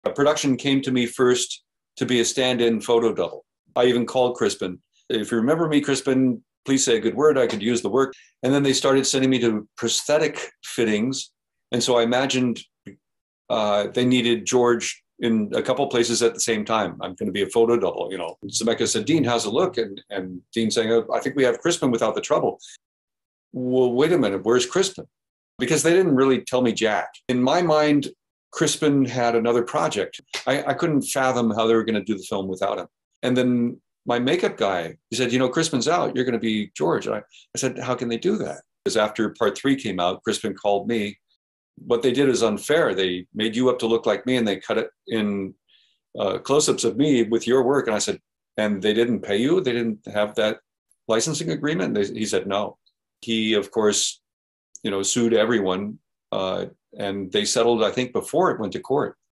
Jeffrey Weissman-replacing Crispin Glover in Back to the Future II & III
Jeffrey Weissman Actor, Director, and Teacher who has appeared in dozens of motion pictures and TV shows, most notably as George McFly in Back to the Future Part II and III and as Teddy Conway in Pale Rider. He has guest starred spots on Scarecrow and Mrs. King, Max Headroom, Dallas, The Man Show, and with Dick Van Dyke on Diagnosis: Murder and as Screech's Guru on Saved by the Bell.